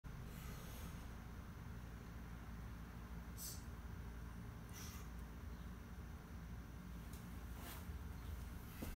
Single Arm KB Press - By Highlander Performance
Equipment: One kettlebell.
1 - Stand with a kettlebell in the front kettlebell rack position (junction of your thumb and first finger in the corner of the kettlebell, elbow tucked to your side).
2 - Take a deep breath, and then squeeze your glutes and core hard.
3 - Press the kettlebell overhead, and exhale hard, locking out at the top.
Note - Excellent for shoulder strength. I personally find it more comfortable than dumbbells or barbells for pressing motions.